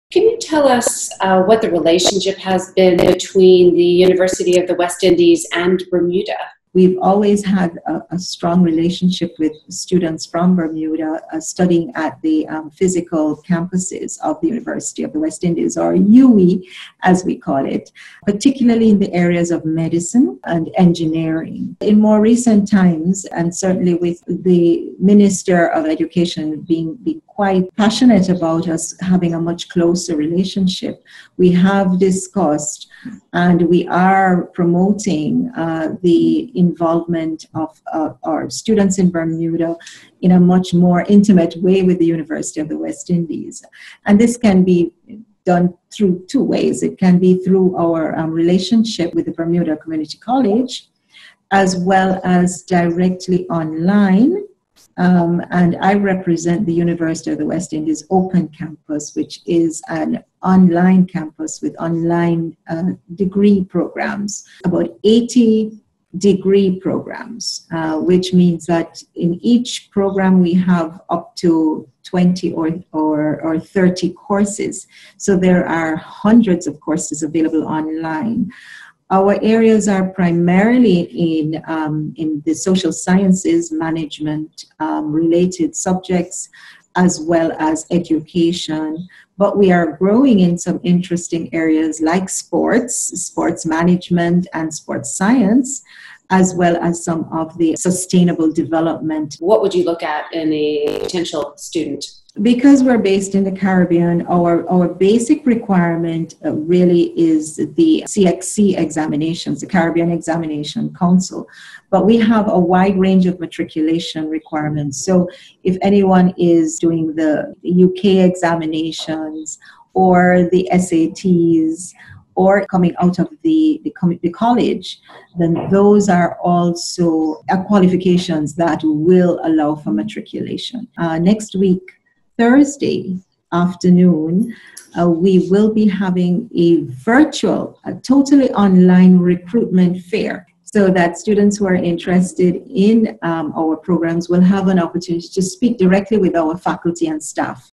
Boosting Qualifications in a post-COVID world
Just to the south of us, in the Caribbean, is an educational gem that some might not be aware of. The University of the West Indies. UWI is ranked in the top 5% of universities worldwide, and shares a member relationship with Bermuda, that affords us discounted pricing.

It could prove especially useful as Bermuda re-builds its economy and, with jobs scarce, young people seek to boost their qualifications.

Dianne Brewer spoke with Pro Vice chancellor and Principal, Dr. Luz Longsworth.